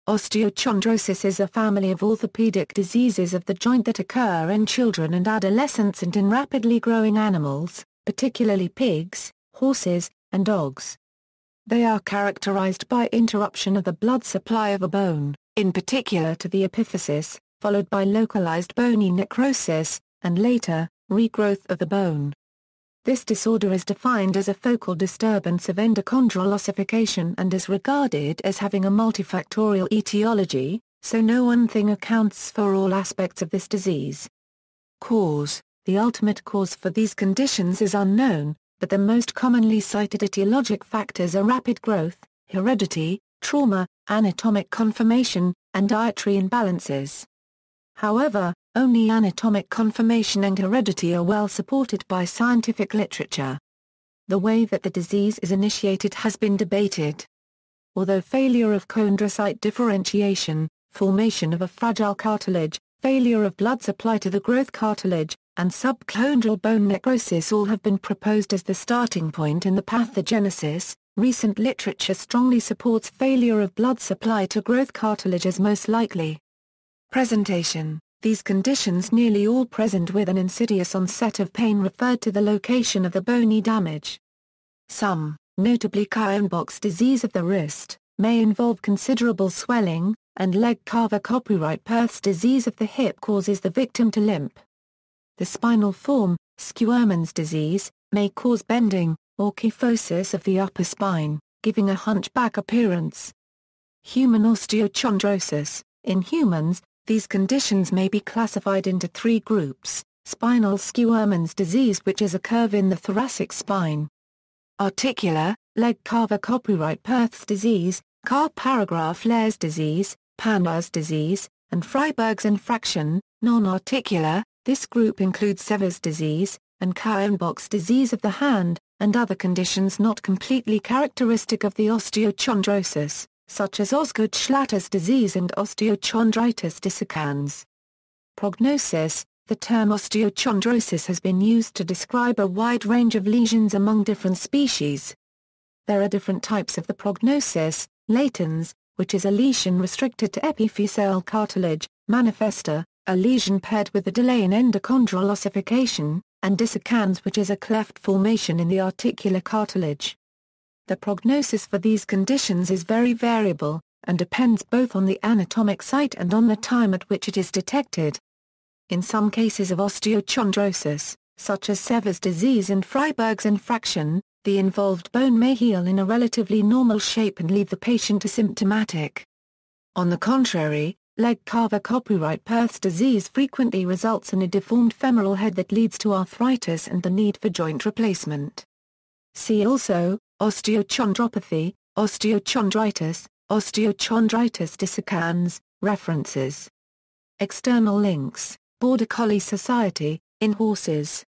Osteochondrosis
Osteochondrosis is a family of orthopedic diseases of the joint that occur in children and adolescents and in rapidly growing animals, particularly pigs, horses, and dogs. They are characterized by interruption of the blood supply of a bone, in particular to the epiphysis, followed by localized bony necrosis, and later, regrowth of the bone. This disorder is defined as a focal disturbance of endochondral ossification and is regarded as having a multifactorial etiology, so no one thing accounts for all aspects of this disease.